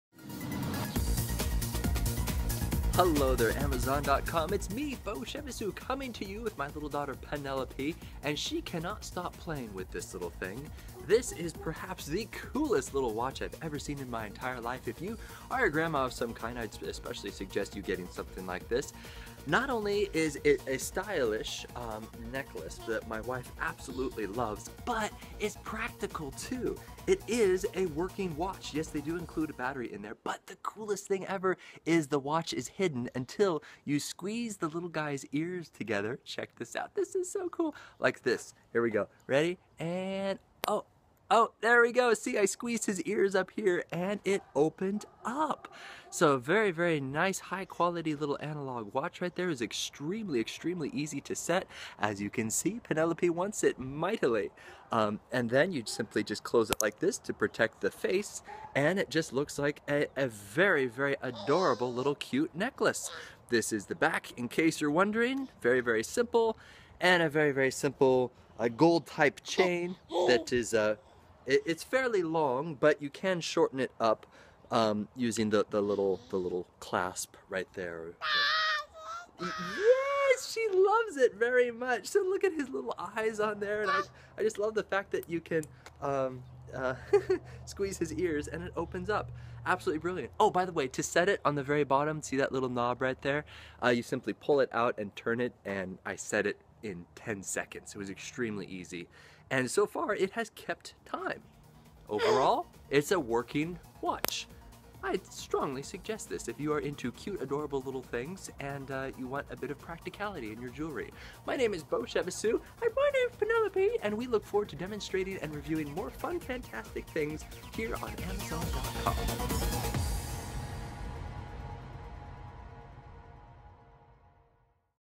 ★★★★★ CSMARTE Pocket Watch- Owl Bronze Steampunk Quartz Watch Pendant with Chain Necklace - Amazon
✏️ Quick thoughts to supplement my video demonstration.

- Push button opens the wings.
- Easy to adjust and set.
- Kids love it when you're holding them. (if you're a grandma)
- Neat steam-punk type design. (if you're into cosplay)
- Overall, super unique... I've never seen anything like this.

Note: Doesn't come with a vase.

Note: I was given a sample of this product in exchange for a review and demonstration, which of course has no bearing on the honesty of my opinions regarding the product. Hope my demonstration and test helps!

👍 Yes, absolutely.